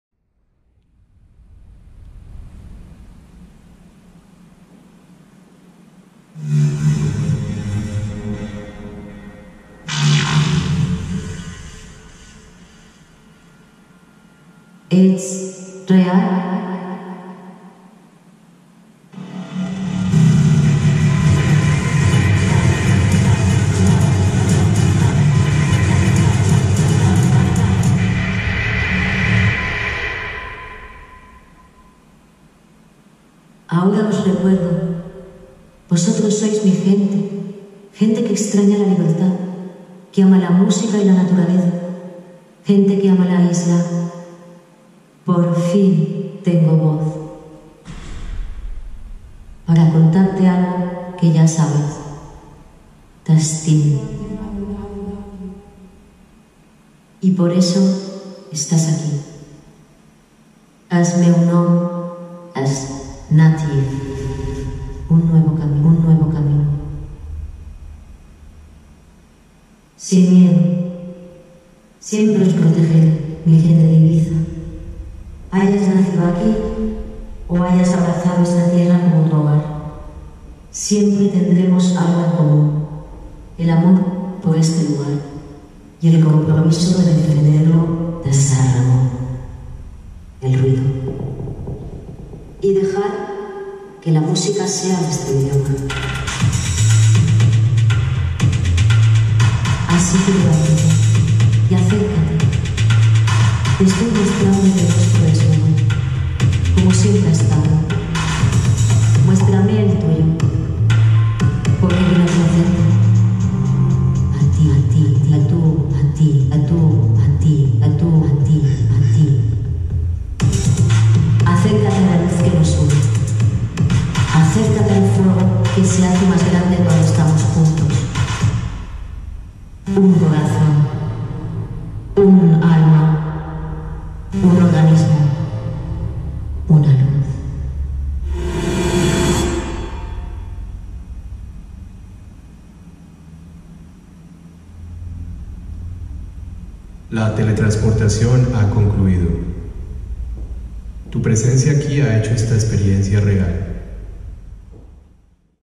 LedPulse Spain presents Teleportas gates of The dragon at Ibermaison Ibiza.
Sound System KonaStorm6.1 by Pequod Acoustics.
Exclusive Distrubutor
HPS IBiza.